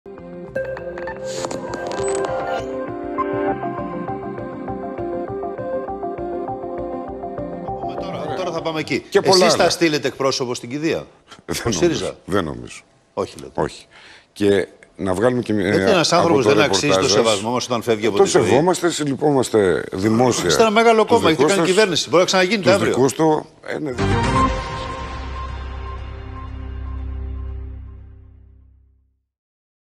Χρήστος Σπίρτζης: Δεν νομίζω να στείλουμε εκπρόσωπο στην κηδεία | Parapolitika
Ο βουλευτής ΣΥΡΙΖΑ, Χρήστος Σπίρτζης σε τηλεοπτικό πάνελ όπου βρέθηκε συλλυπήθηκε την οικογένεια του τέως βασιλιά για την απώλεια, ωστόσο δήλωσε πως ο ΣΥΡΙΖΑ κατά πάσα πιθανότητα δεν θα στείλει εκπρόσωπο στην κηδεία.